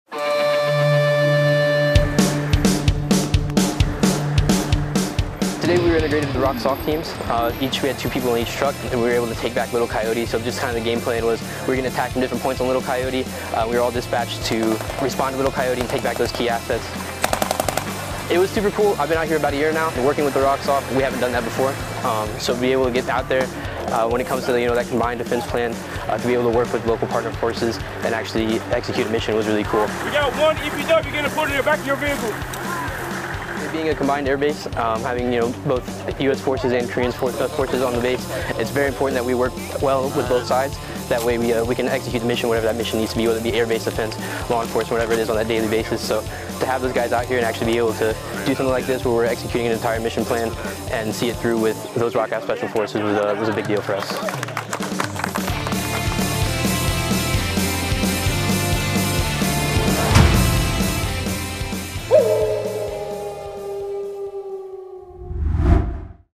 Strengthening Alliance: Republic of Korea & U.S. Air Force unite for joint training
Republic of Korea Army soldiers and, U.S. Air Force 8th Security Forces Squadron Defenders train together, increasing interoperability and strengthening their alliance at Kunsan Air Base, ROK, May 18, 2023.

Protect the Pack
SOUTH KOREA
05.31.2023

In bilateral training events, U.S. and ROK service members are able to share tactics and sharpen each other’s skills. (U.S. Force video by Senior Airman Karla Parra)

Film Credits: Video by Senior Airman Karla Parra
8th Fighter Wing Public Affairs